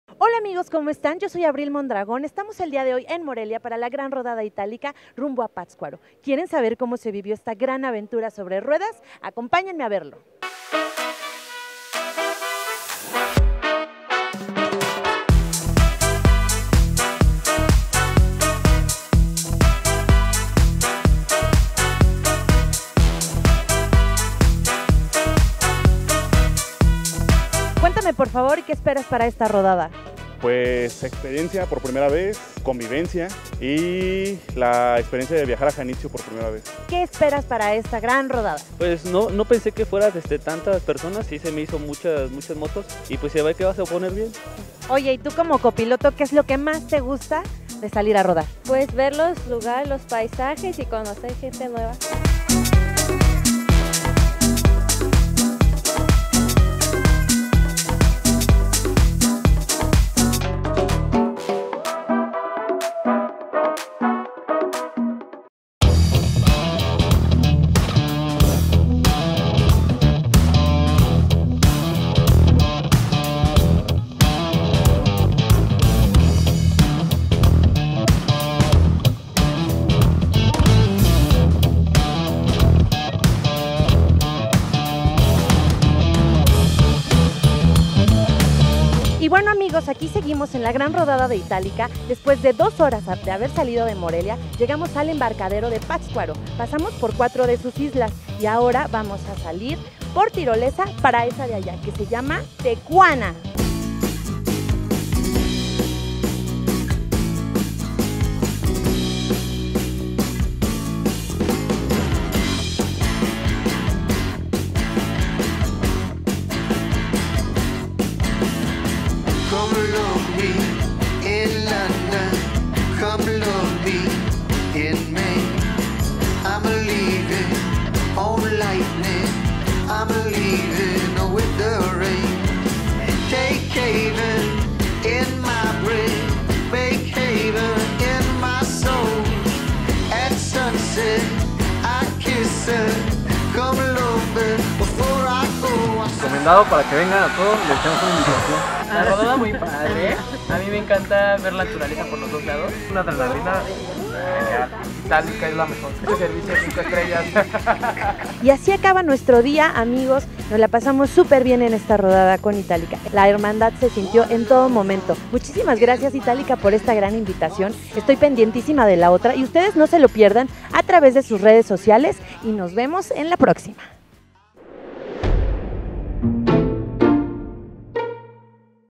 DE MORELIA A PATZCUARO - RODADA ITALIKA
En esta ocasión  tuvimos la oportunidad de rodar con nuestros amigos de Morelia a Patzcuaro y conocer sus islas.